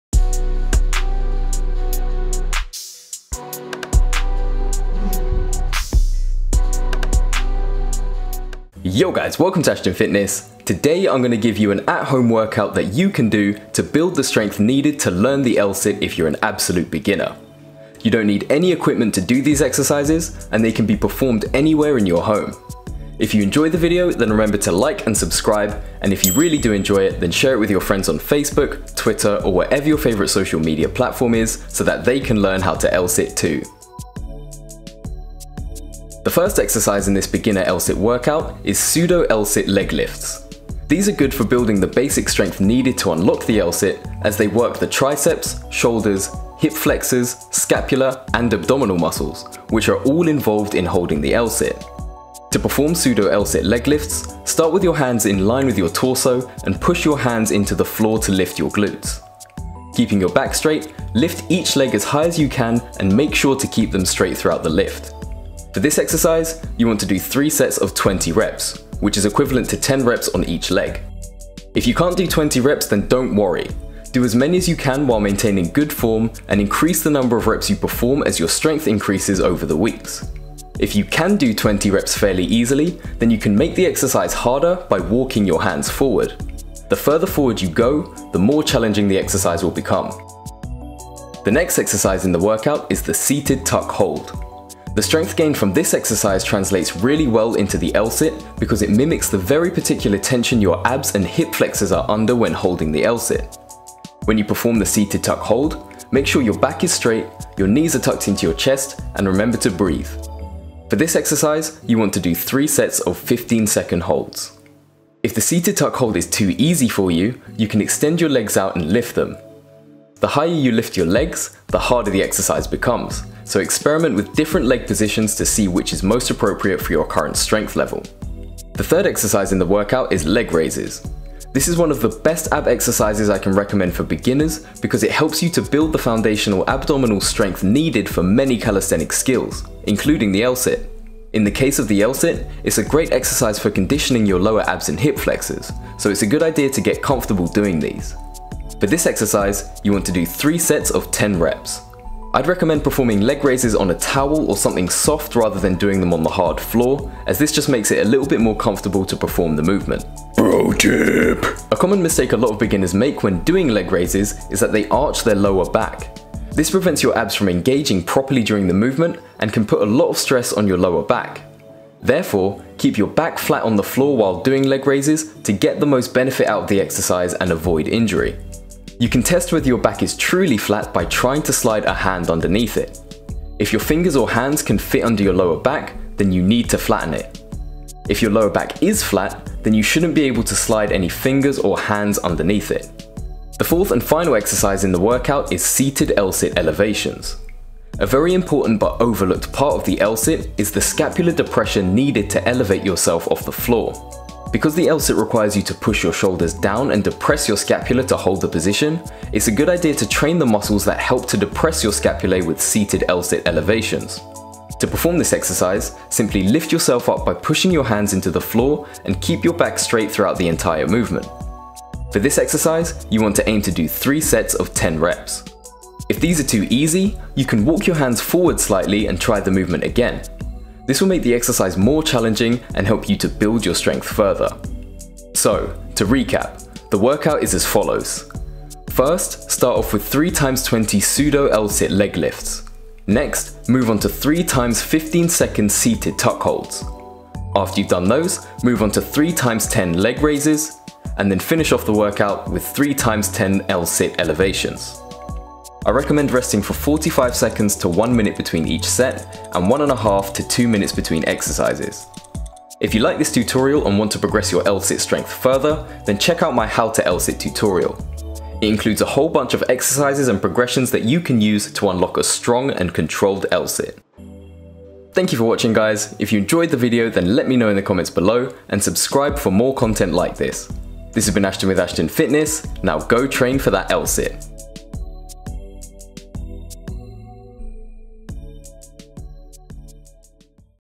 Home L-Sit Workout For Absolute Beginners | NO EQUIPMENT (Calisthenics for Beginners)
Here's a great home workout for beginners who want to learn the L-sit!

If you've always wanted to learn how to L-sit but didn't know where to start, this is the perfect video for you.

I created this home L-sit workout for calisthenics beginners who have just started their calisthenics training and want to build the basic strength needed to perform the L-sit.

It contains a range of exercises that target all the key muscles involved in holding the L-sit (such as the abs, triceps, and hip flexors) while still being accessible to those who have just started their calisthenics journey.

The best thing about this calisthenics home workout is that it can be done with no equipment, so you can do it literally anywhere!

If the exercises in this video are too easy for you, or if you want to take your L-sit training further, then watch my full L-sit tutorial:

Have fun with your L-sit workout, and happy training!

*Easter egg for anyone who reads this far down*: I actually made the beat for this video :D hehehe hope you like it